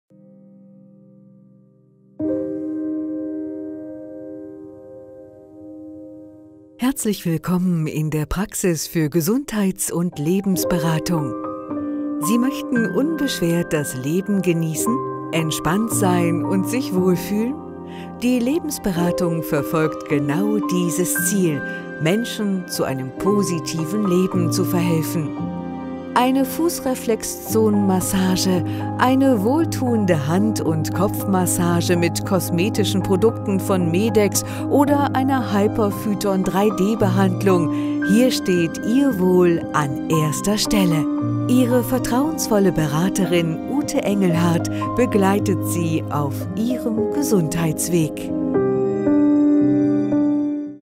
Selected Pharma - Ihr Wohl an erster Stelle
Herzlich willkommen in der Praxis für Gesundheits- und Lebensberatung.
Sie möchten unbeschwert das Leben genießen? Entspannt sein und sich wohlfühlen? Die Lebensberatung verfolgt genau dieses Ziel: Menschen zu einem positiven Leben zu verhelfen!
Eine Fußreflexzonenmassage, eine wohltuende Hand- und Kopfmassage mit kosmetischen Produkten von Medex oder eine Hyper Phyton 3D Behandlung – hier steht ihr Wohl an erster Stelle.